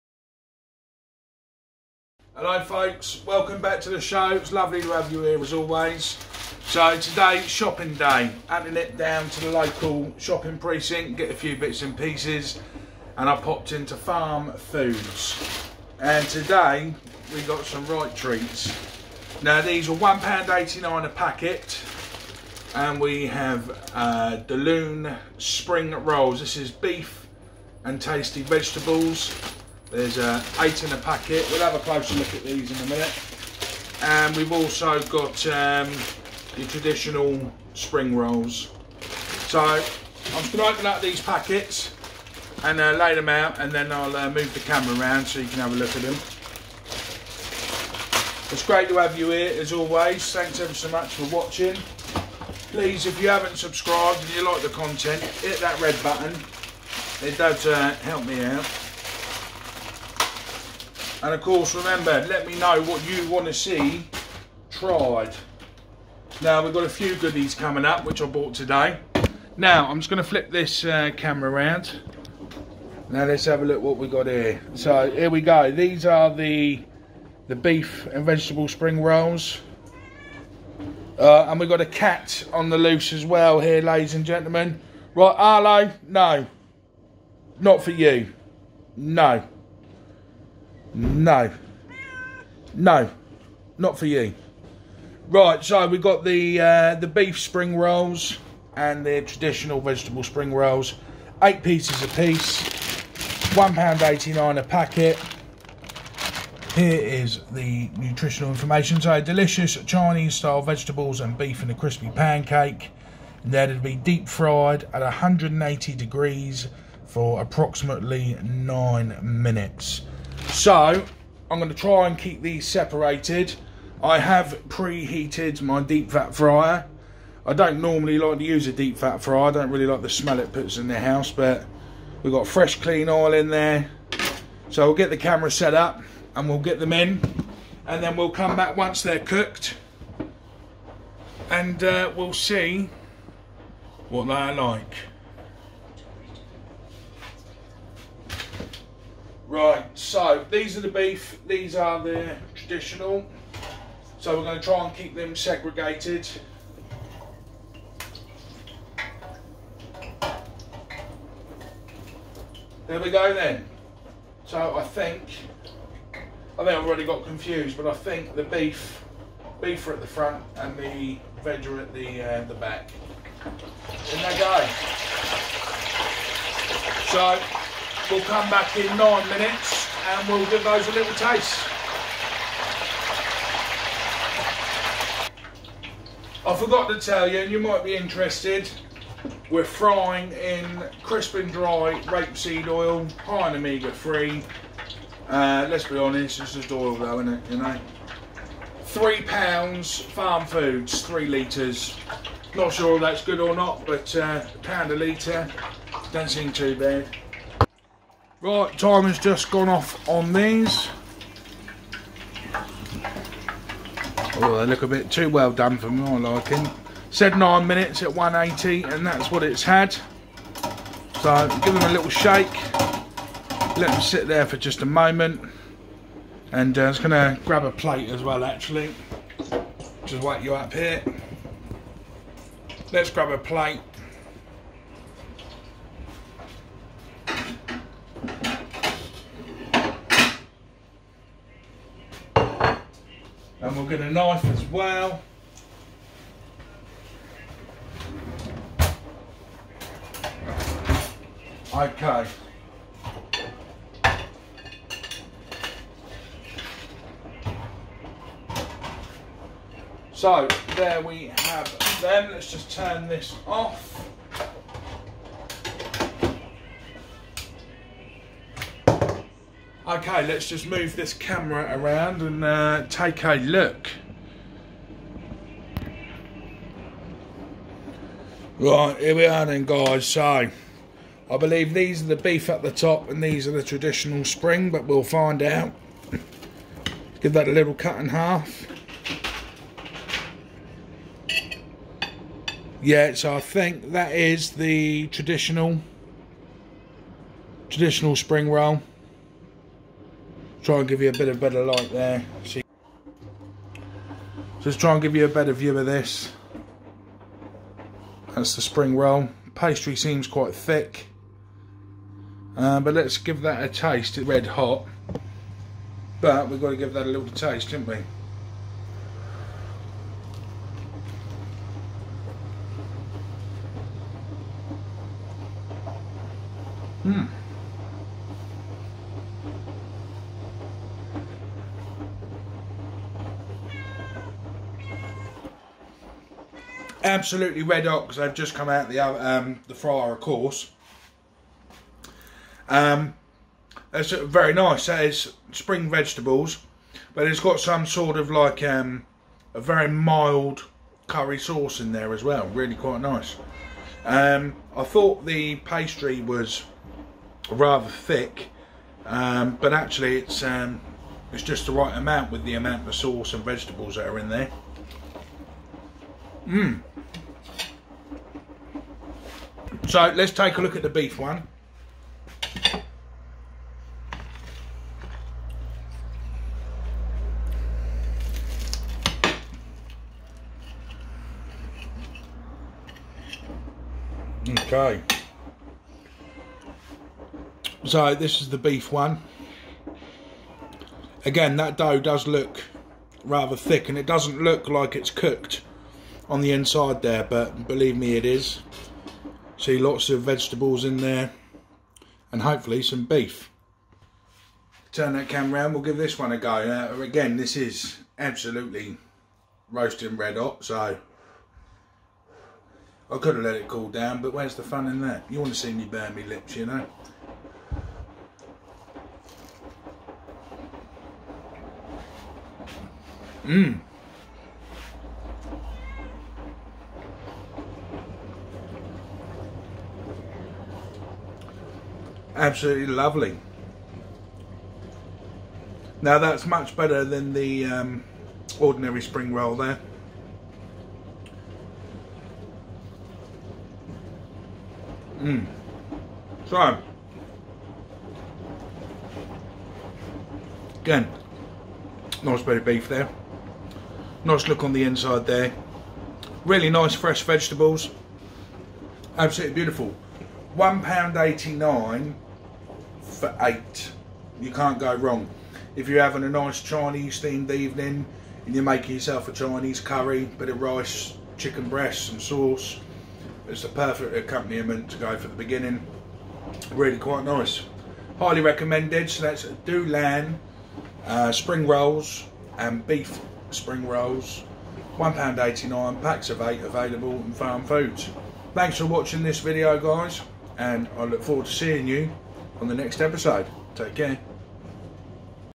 Beef & Vegetable Spring Rolls - Farmfoods
£1.89 For a pack of 8 Spring Rolls - Available at Farmfoods Nationwide